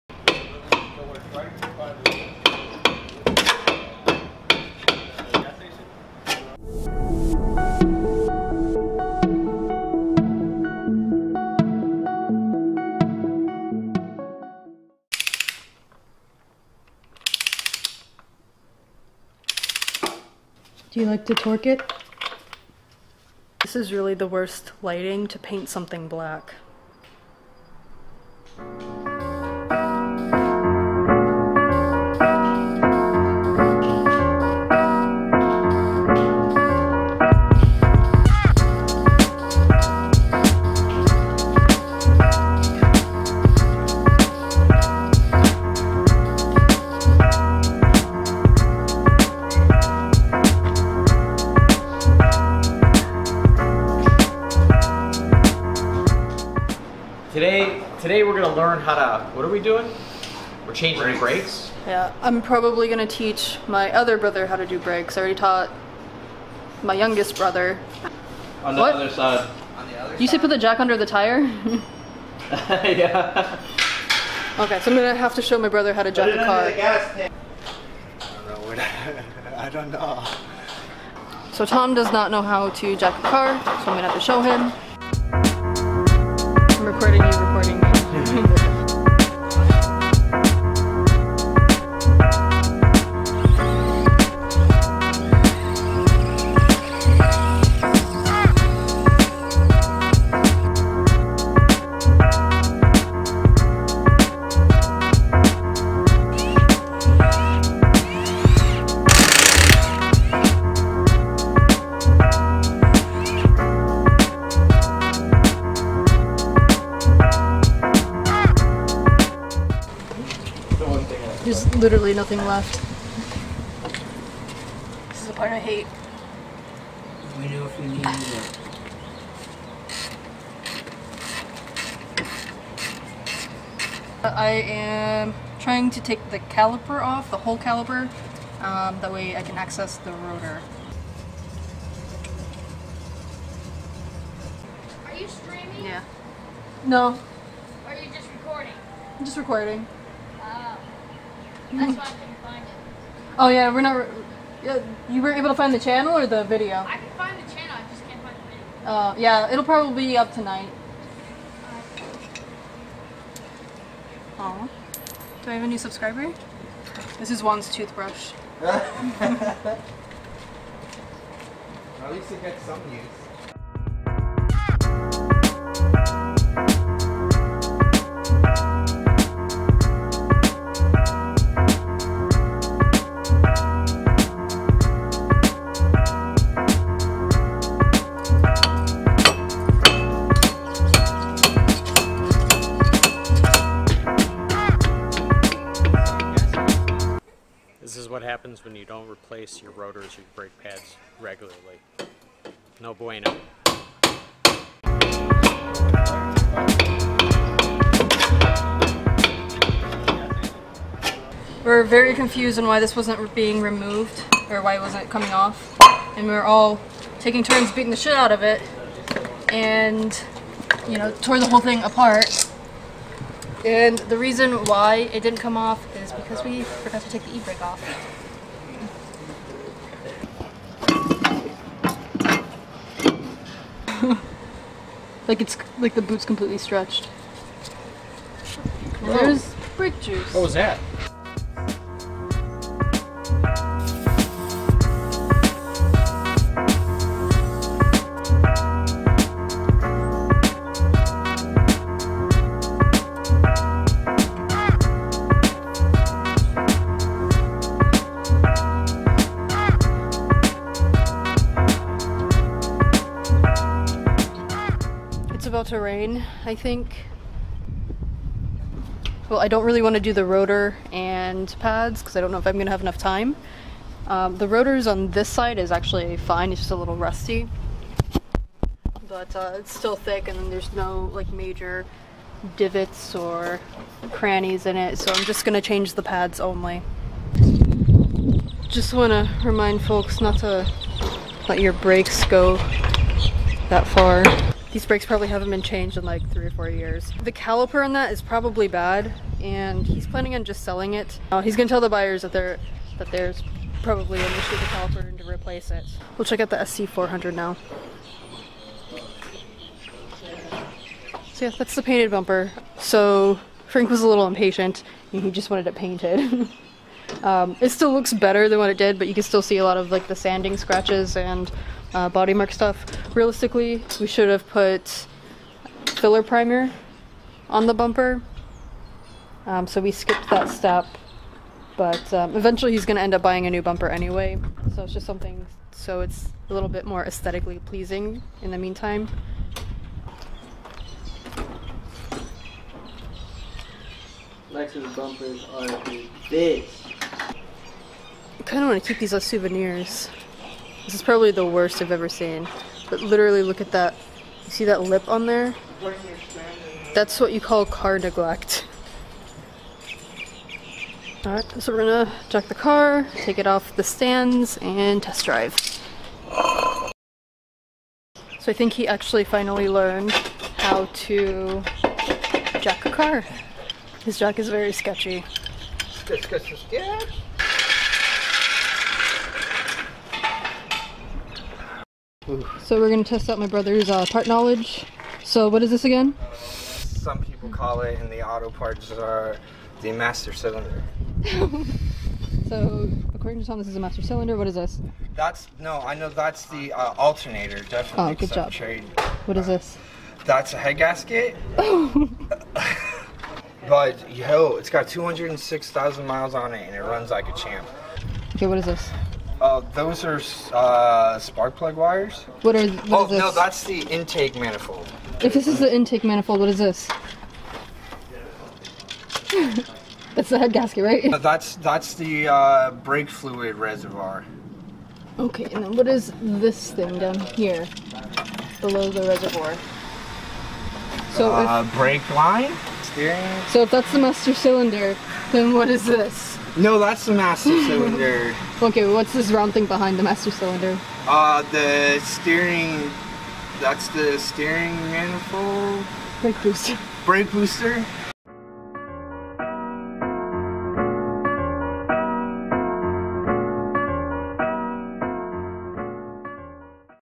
How many cats does it take to remove a rotor?
Weekend spent at the shop. Trying to teach my brother a thing. Learning the settings on my new camera.